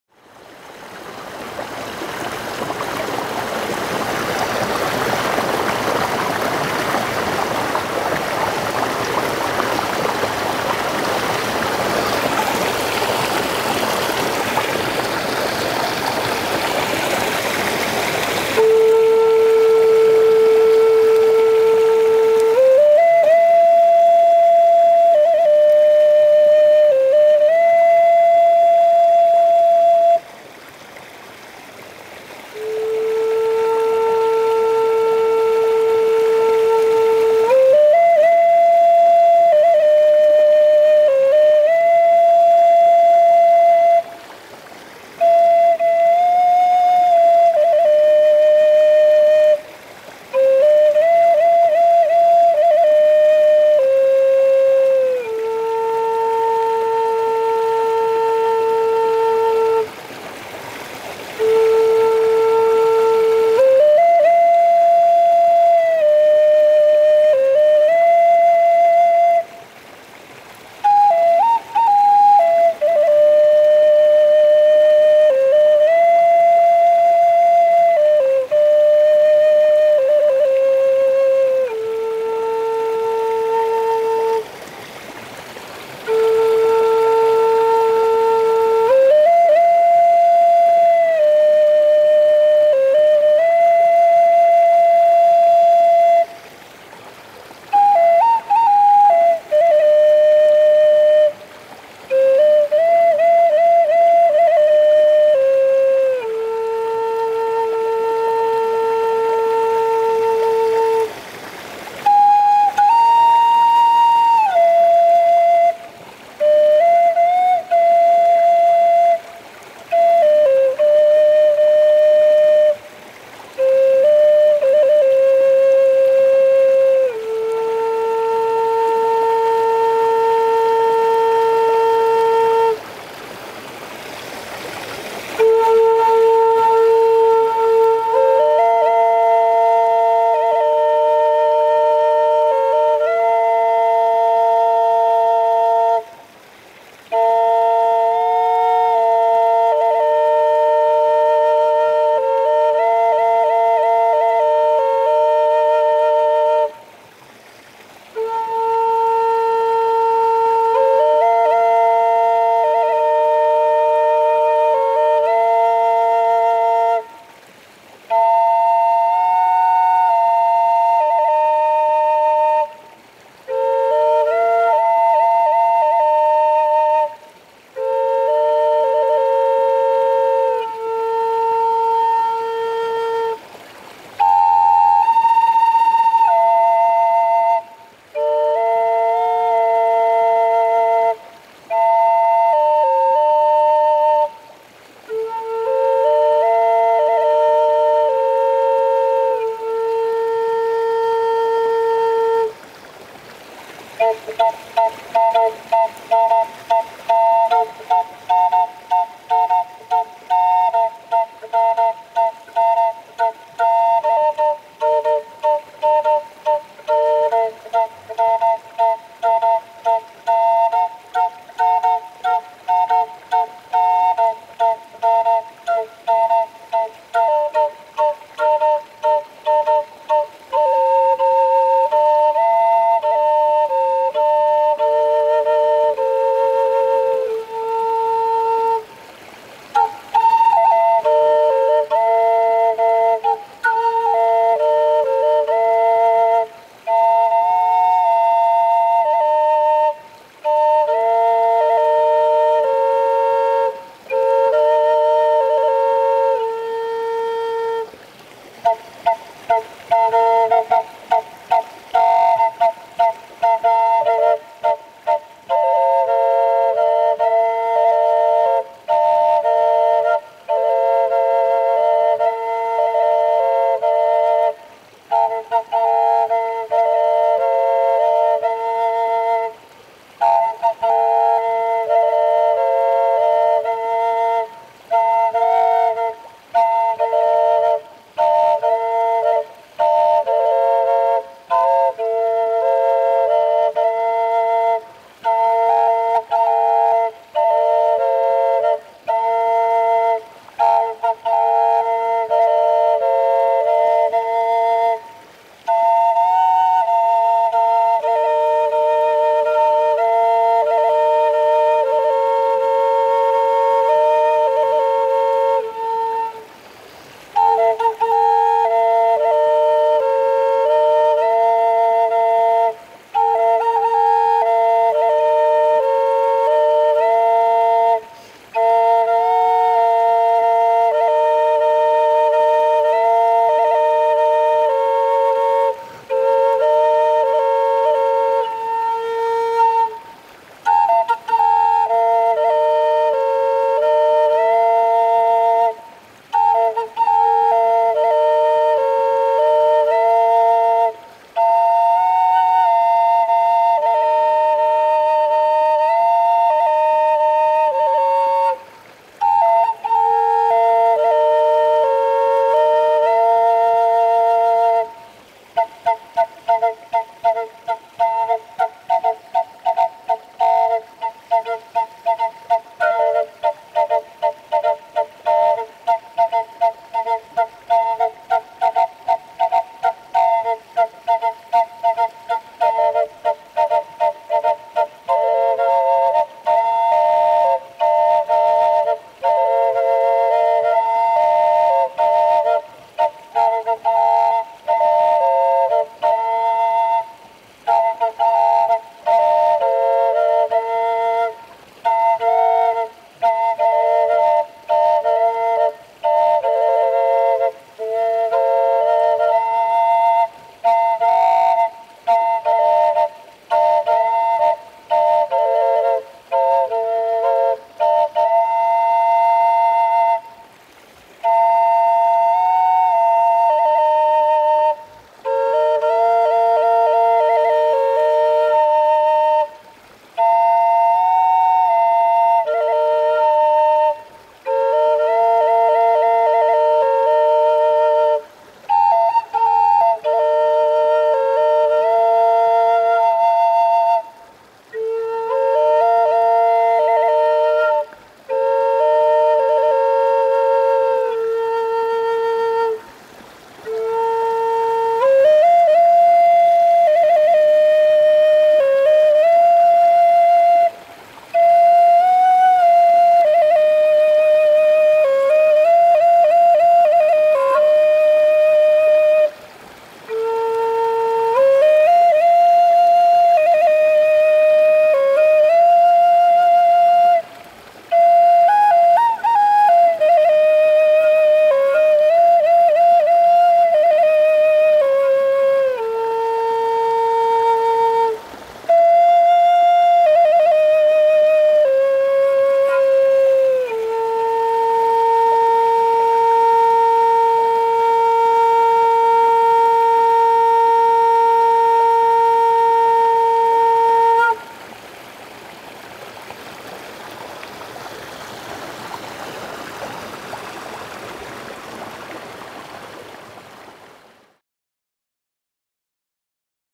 Triple Flute am Untersberg von Christopher Amrhein 2020 08 27
Die Triple Flute von High Spirit ist ein magisches Instrument. Ich liebe sie an magischen Plätzen wie den Untersberg zu spielen. Viel Freude beim Hören und Sehen. Christopher